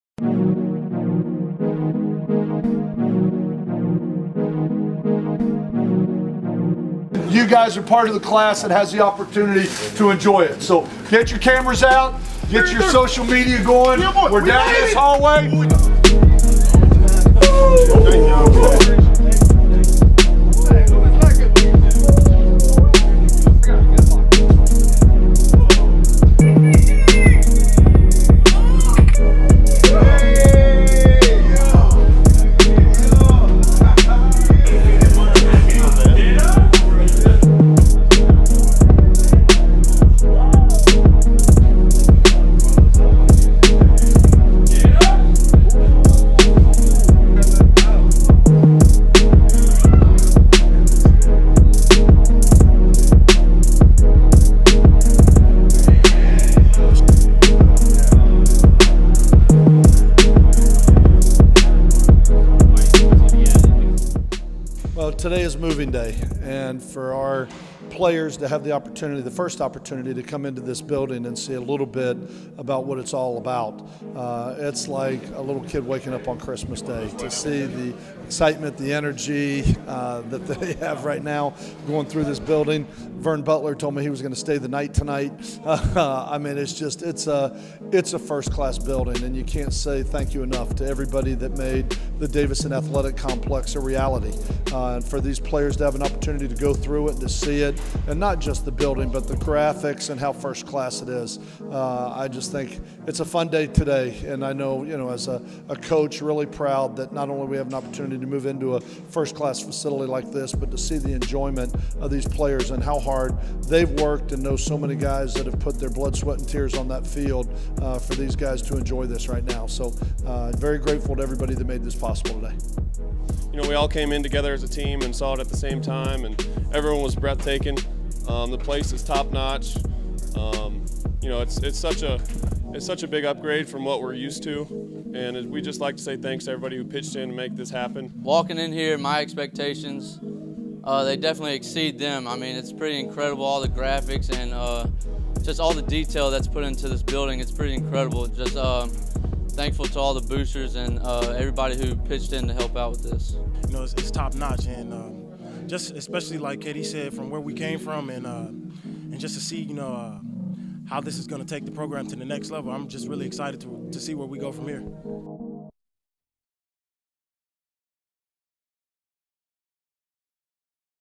Davison Athletics Complex Reveal
LA Tech Athletics unveils the new Davison Athletic Complex.